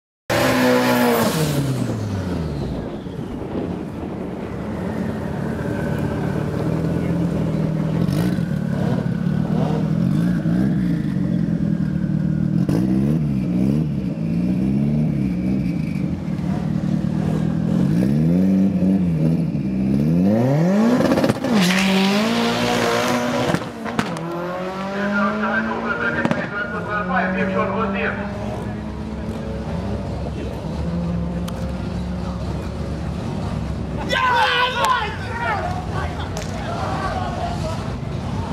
March 2016 QTM Day 3 Run 09
13th March 2016 - Day 3 "Eliminations and Finals" of the First Quarter Mile Weekend for the 2016 Championship. The event was organised by the MDRA and held at Hal Far Raceway.